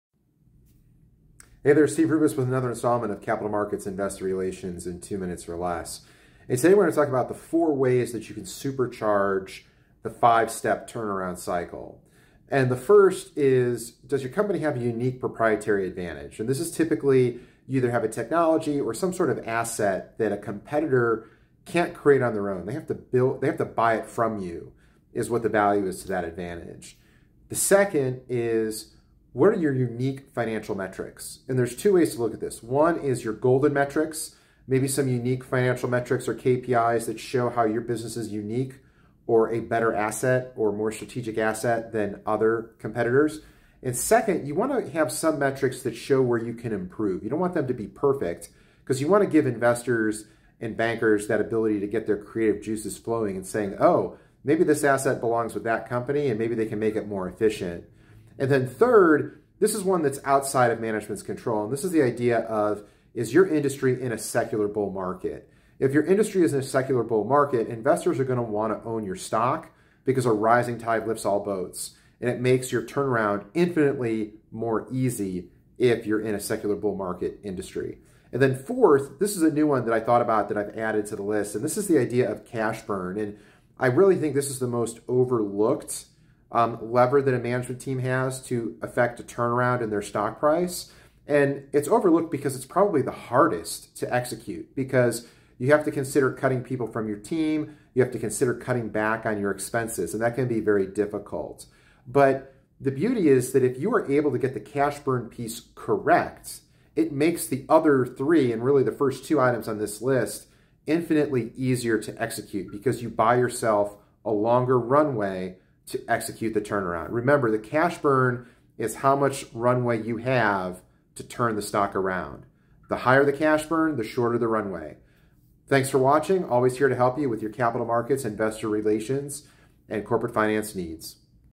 Four Ways to Supercharge the Rubis Five-Step Turnaround Cycle
Management can supercharge the turnaround of a sub $5 stock with the following:
1. Unique Proprietary Advantage
2. Unique Financial Metrics
3. Secular Bull Market
4. Cash Burn